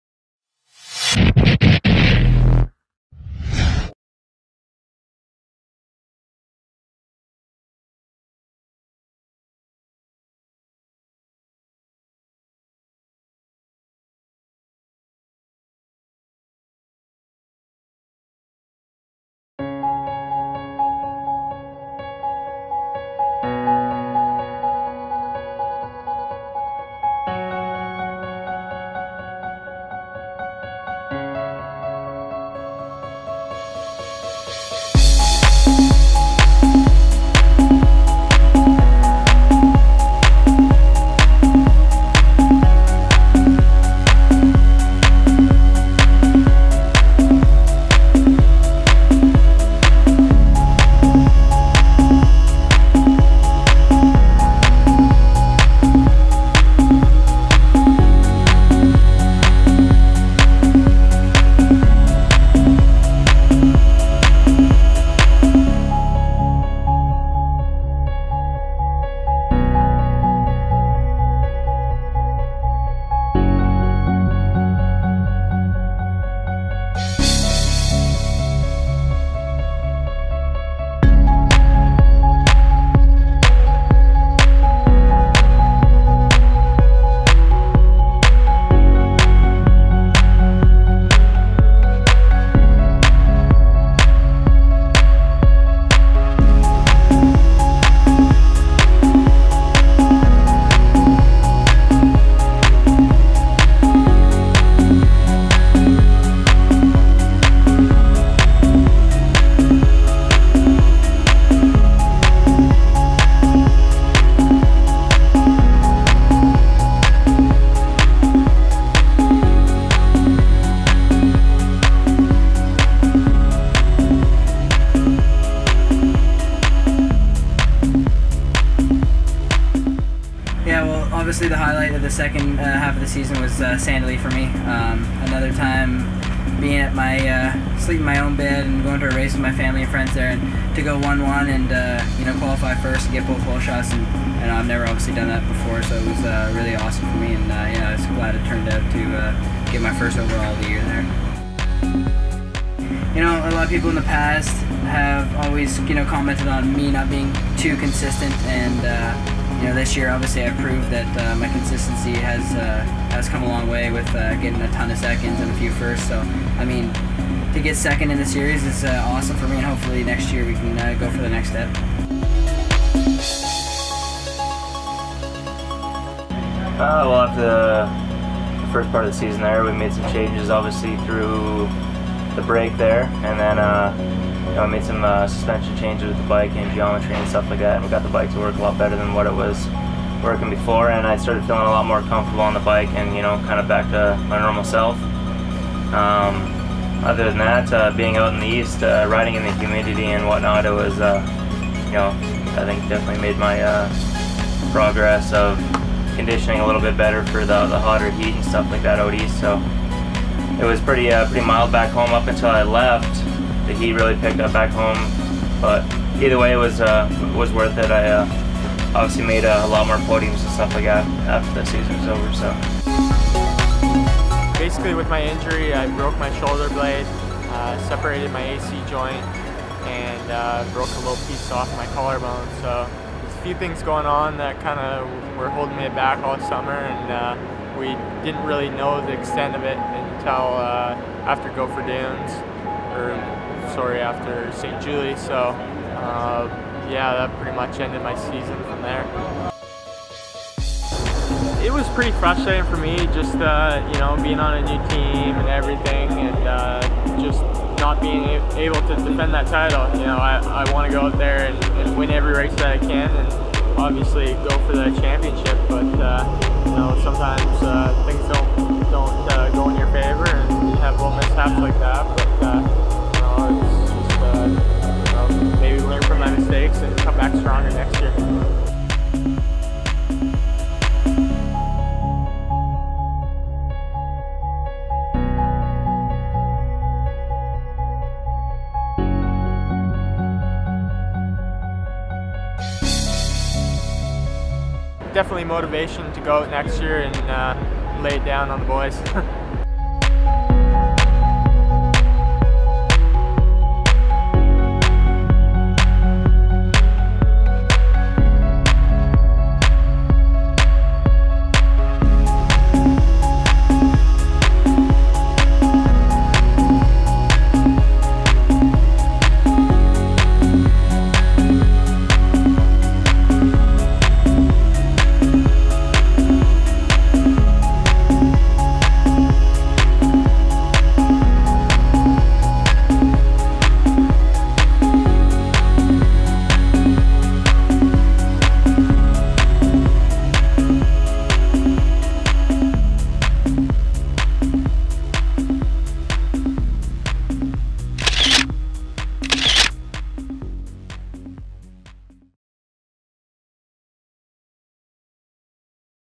Part II: KTM Canada MX season review 2012
A look back at the second half of the Canadian Motocross Nationals, featuring the KTM Red Bull Royal Distributing Fox Racing Team (and their riders - Colton Facciotti, Dusty Klatt and Jeremy Medaglia).

Video produced by James Lissimore (lissimorephoto.com).